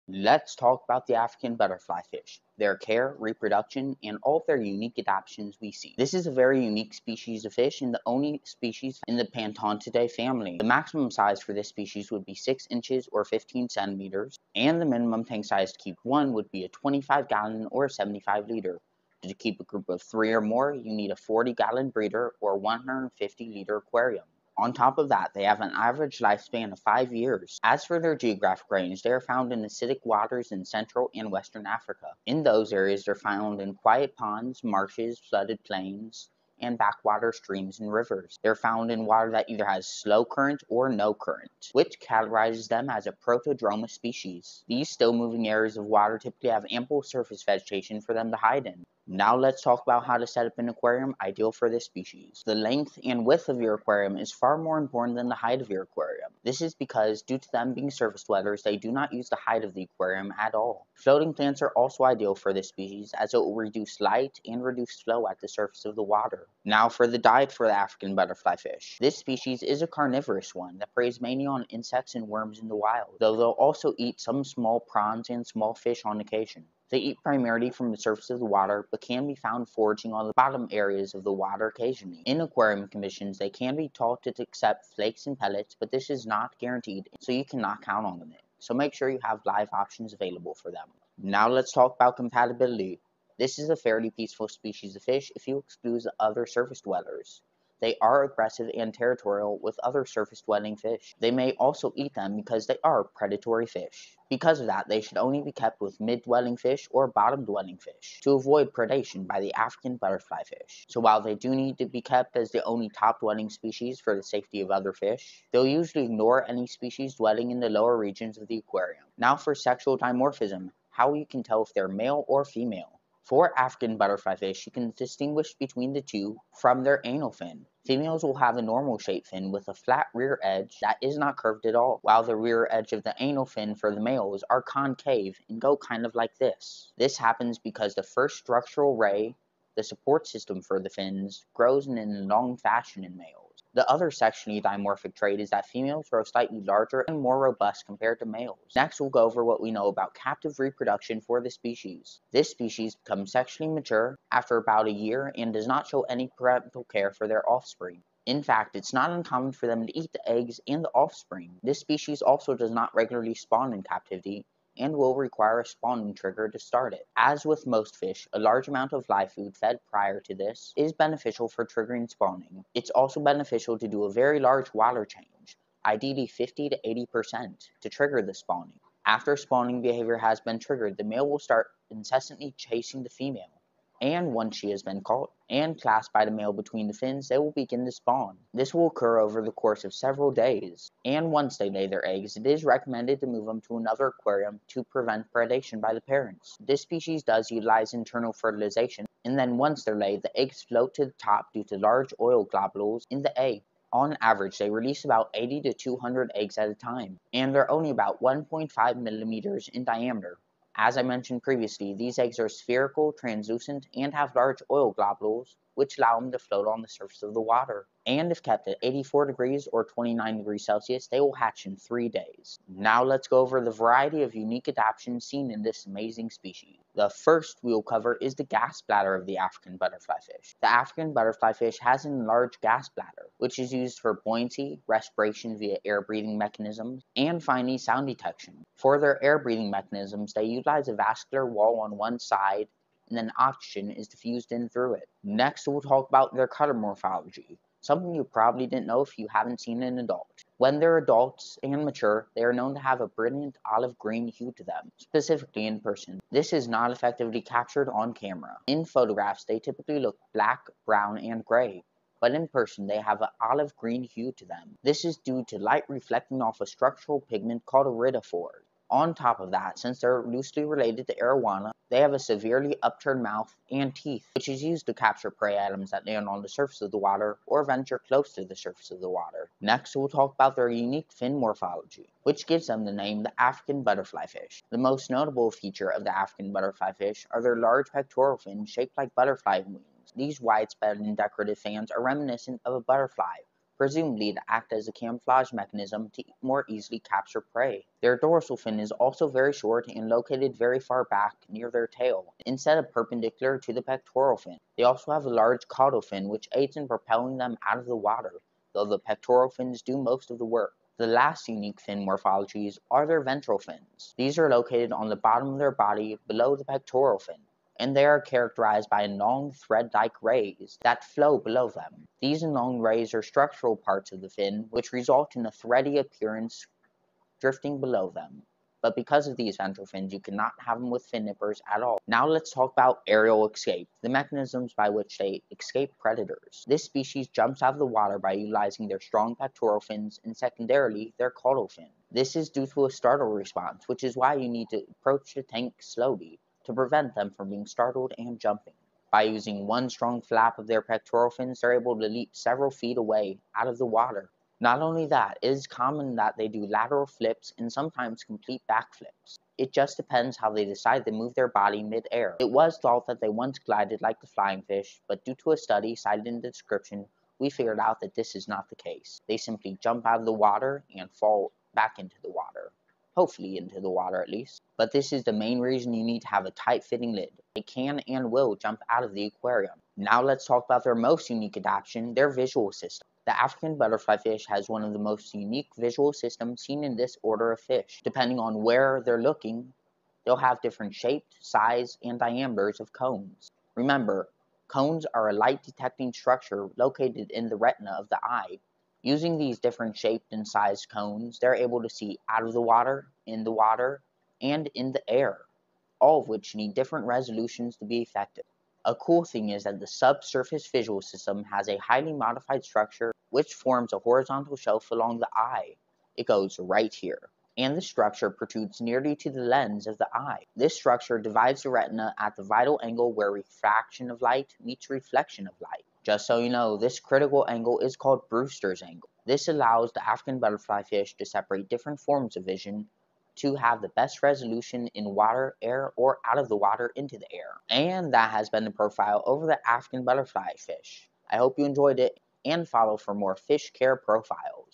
Cited African Butterfly Fish: Care, Reproduction, Unique Adaptions
Citations

Care information

Size, age, compatibility,

Ibim, A. T., and J. O. Ike. "Aspects of the biology and culture of the butterfly fish, Pantodon buchholzi; A potential aquarium fish in Nigeria." Journal of Applied Sciences and Environmental Management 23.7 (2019): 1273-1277.

Gad bladder

Icardo, José M., et al. "The gas bladder of Pantodon buchholzi: Structure and relationships with the vertebrae." Journal of Morphology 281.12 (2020): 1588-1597.

Aerial escape

Saidel, William M., Gabriel F. Strain, and Shannon K. Fornari. "Characterization of the aerial escape response of the African butterfly fish, Pantodon buchholzi Peters." Environmental Biology of Fishes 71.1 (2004): 63-72.

Nervous and visual system

Saidel, William M. "Coherence in nervous system design: the visual system of Pantodon buchholzi." Philosophical Transactions of the Royal Society of London. Series B: Biological Sciences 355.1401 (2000): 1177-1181.